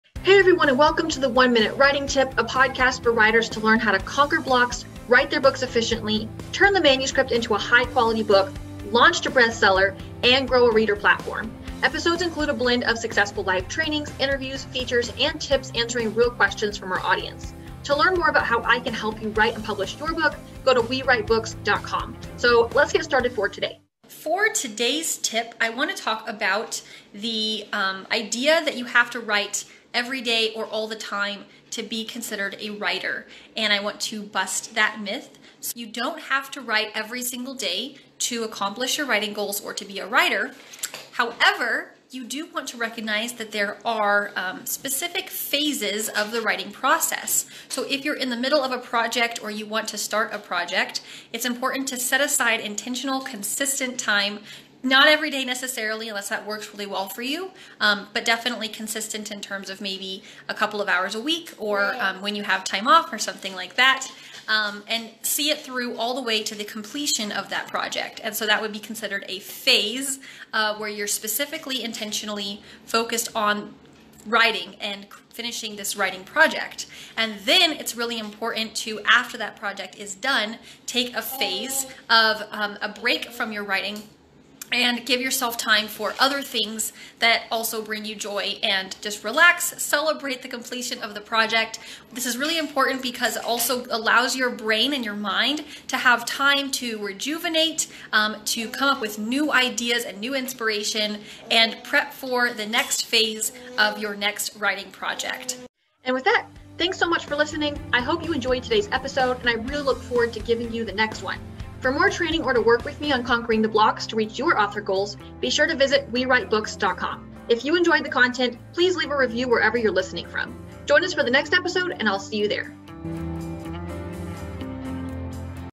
You Don't Have to Write Every Day to Be a Writer - 1 Minute Writing Tip #11
Do you feel like you're not a writer because you don't write every day?

In this episode, I'm busting this myth and sharing what is required to be a writer and how to have the right focus with your writing.

Ready to conquer the blocks and finally write and publish your book?